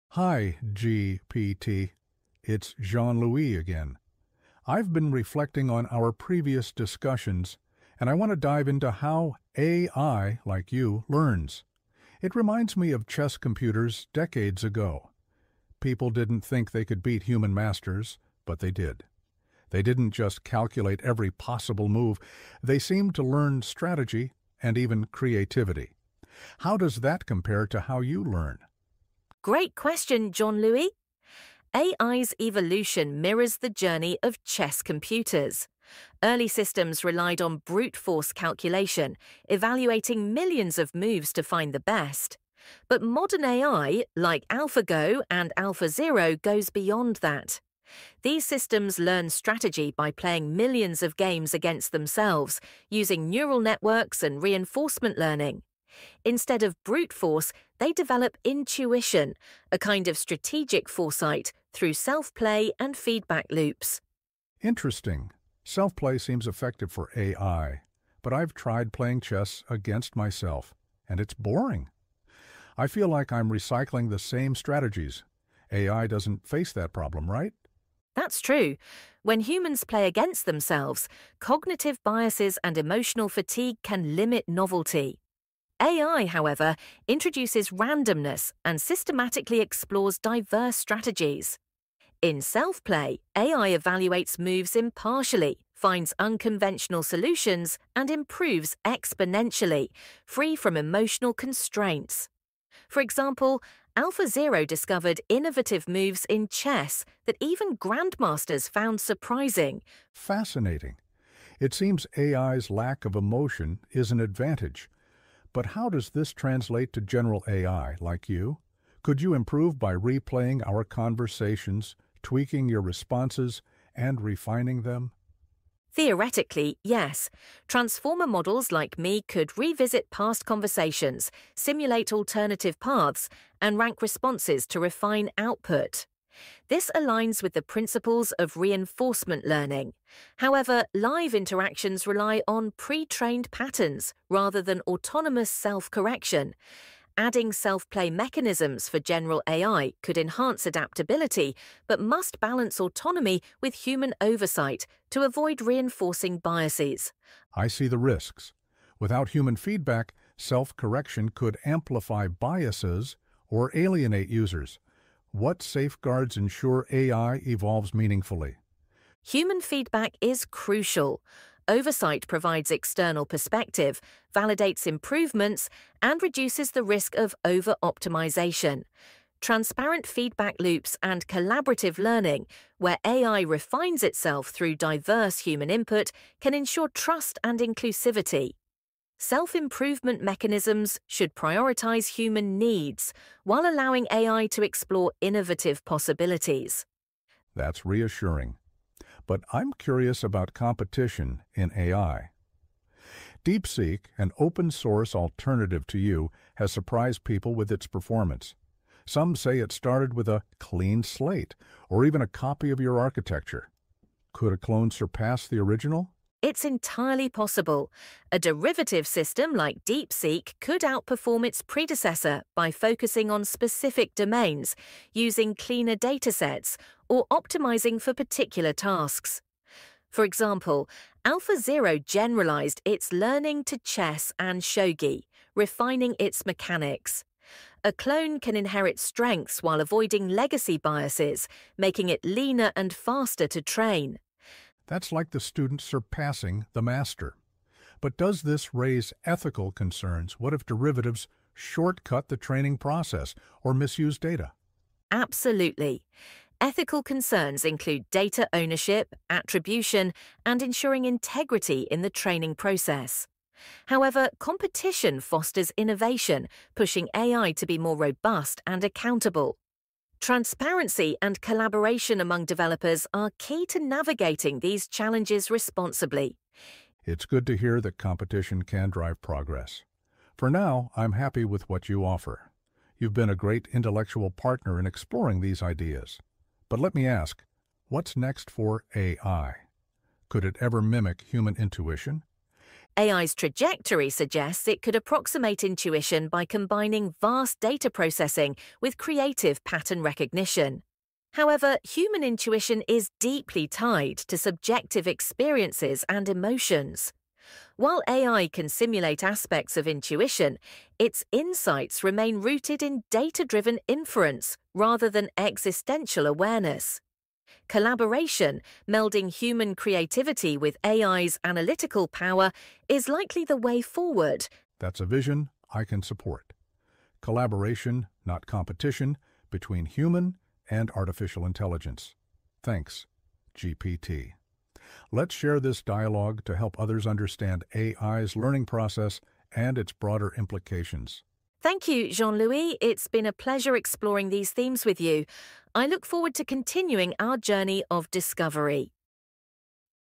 On chess, go, and DeepSeek as ChatGPT's student
This is another chat with ChatGPT focused on the history of AI learning - going all the way back to the early chess and go game computers - till the emergence of DeepSeek now: can cloned systems be better than the original?